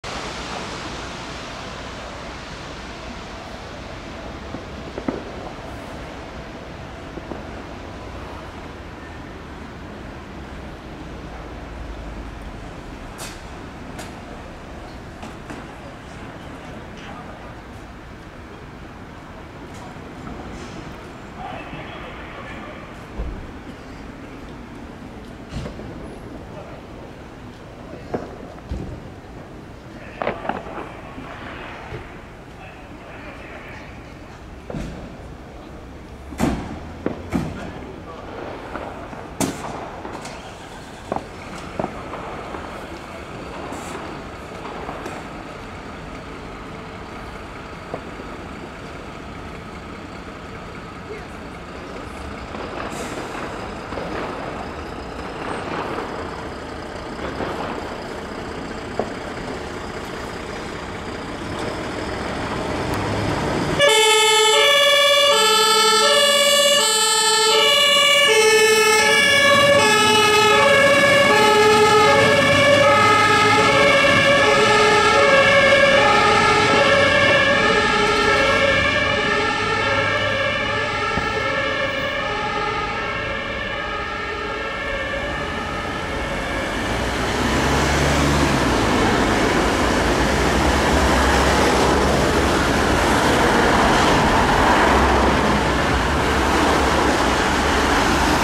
HLF 20 BF Wiesbaden FW 1 Innenstadt
Einsatzfahrten der Einsatzkräfte aus Wiesbaden an Silvester 2018/2019.

Quelle: RheinMainTaunus112

Info

Datum:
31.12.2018

Genaue Örtlichkeit:
Wiesbaden

Distanzierung: folgt!